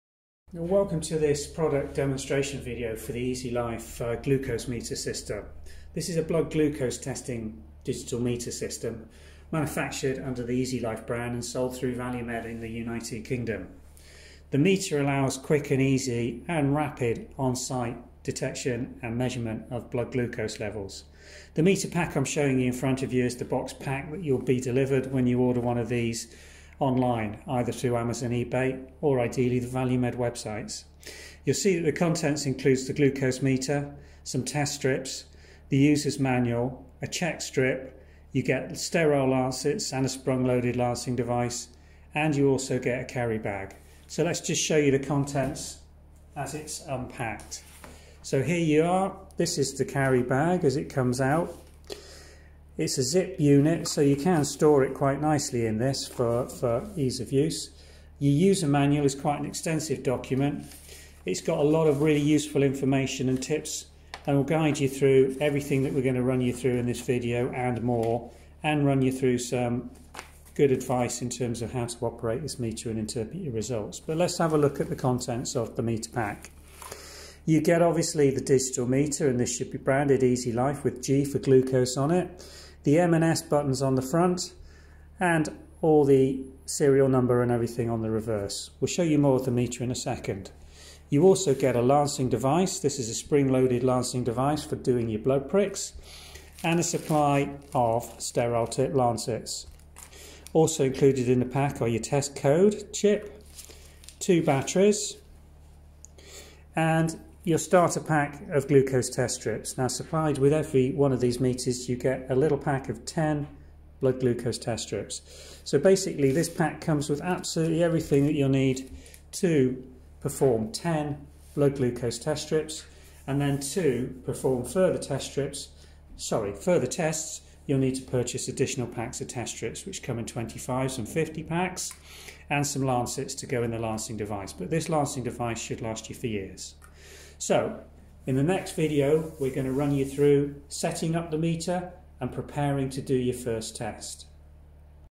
How to use an EasyLife Glucose Meter part 1
This video shows what you get in the starter pack of the EasyLife blood glucose meter system. We show you the pack contents so you understand exactly what is included in the offer.
In later videos in the product series we will demonstrate setting up the meter with time and date, so you are ready to perform your first glucose test with full memory function.